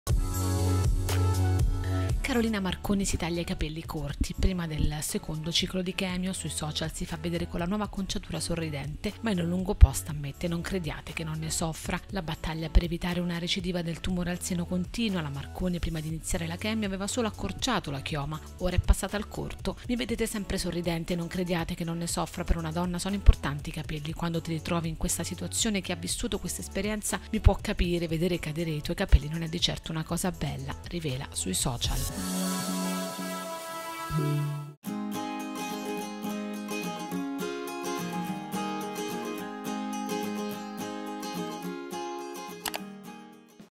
Carolina Marconi accorcia ancora di più i capelli: "Non è facile accettare di perderli"
Carolina Marconi si taglia i capelli corti prima del secondo ciclo di terapia. Sul social si fa vedere con la nuova acconciatura sorridente, ma in un lungo post ammette: “Non crediate che non ne soffra” e l'ex gieffina spiega che...